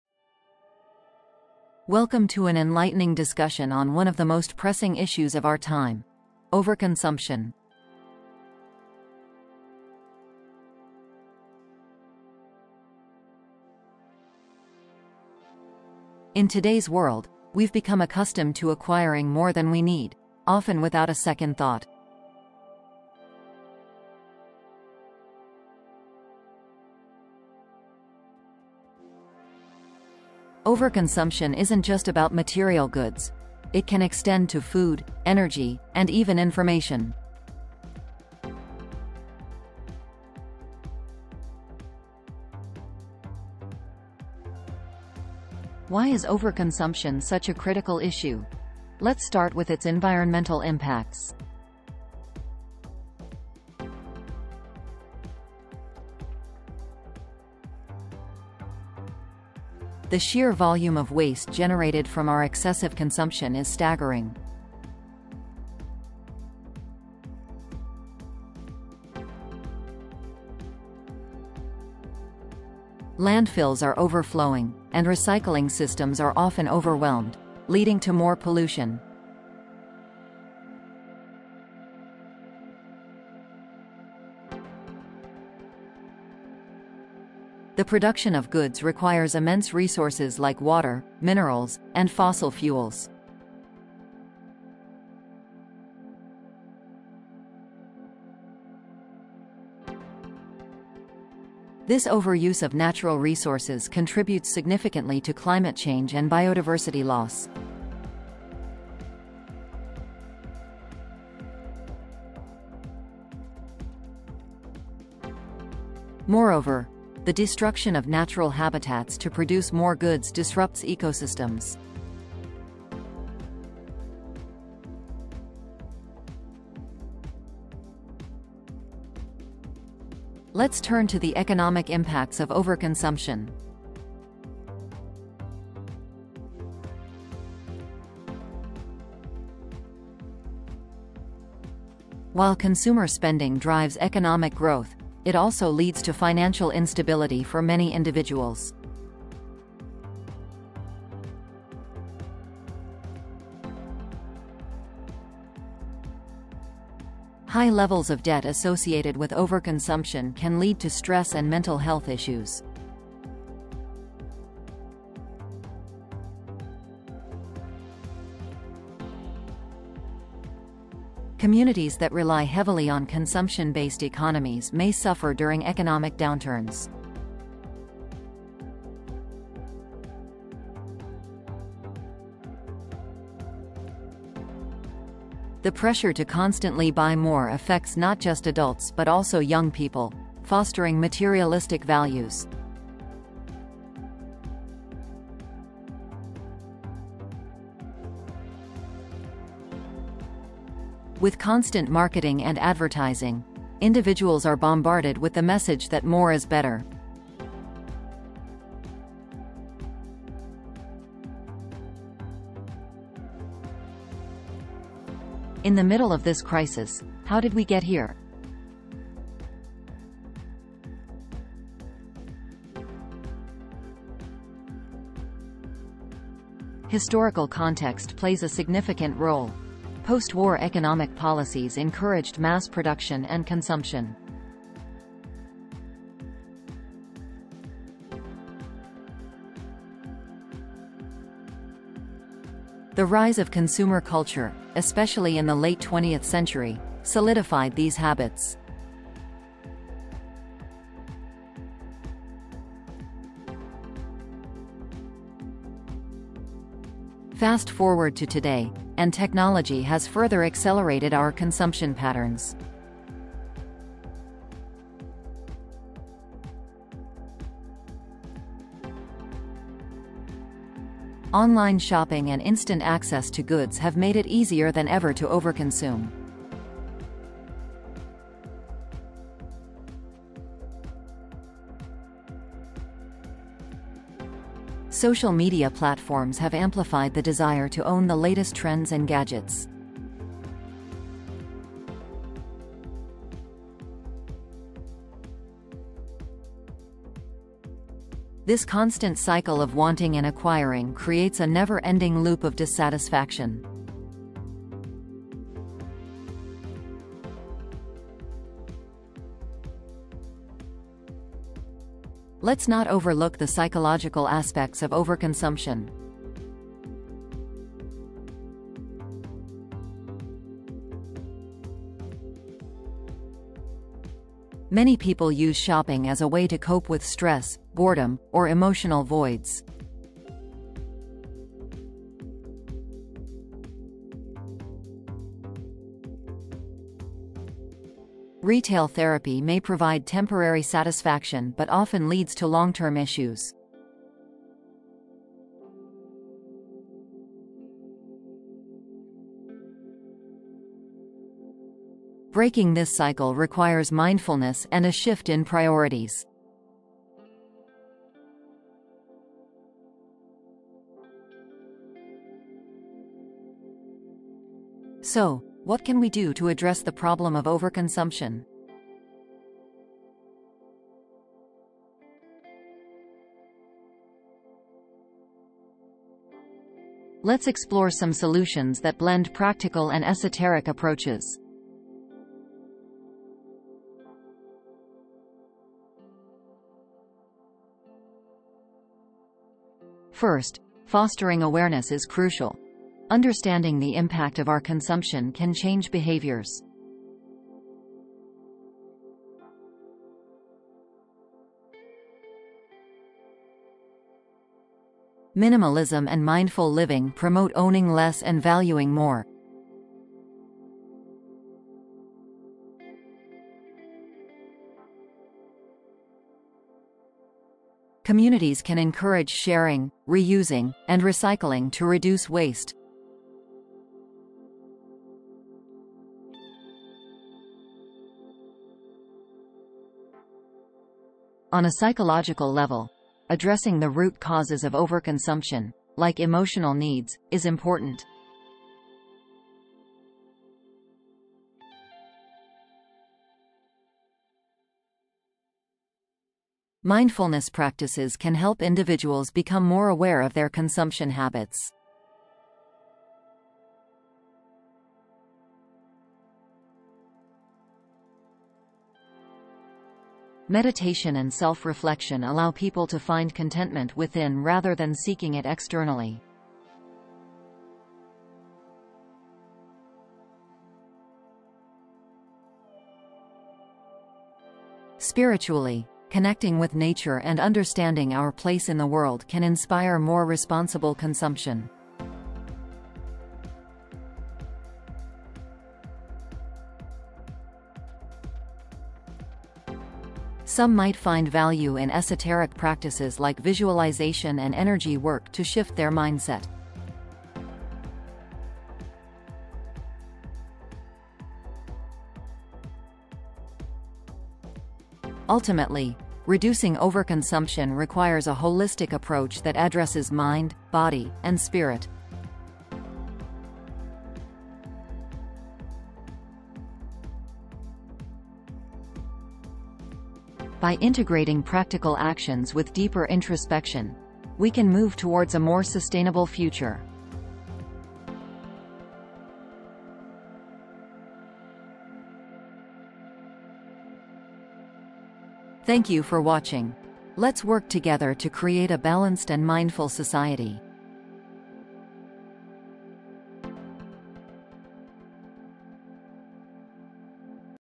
The Modern Epidemic: Overconsumption and Its Impact on Society
Dive deep into the widespread issue of overconsumption and its alarming impact on our society. Understand how we reached this point and discover a unique approach to addressing the problem.